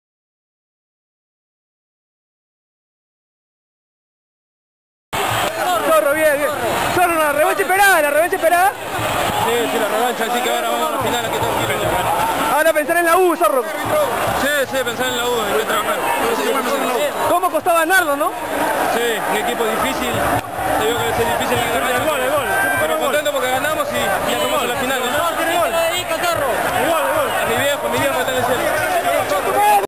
José Gálvez FBC 0 - 1 Alianza Lima / Wilmer Aguirre
Desde Chimbote, las declaraciones del goleador del partido.